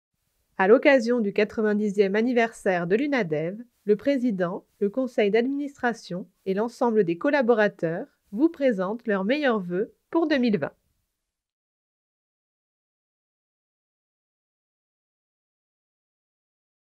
Vœux UNADEV 2020
L’UNADEV, le Président, le conseil d’administration et l’ensemble des collaborateurs vous présentent leurs meilleurs vœux pour 2020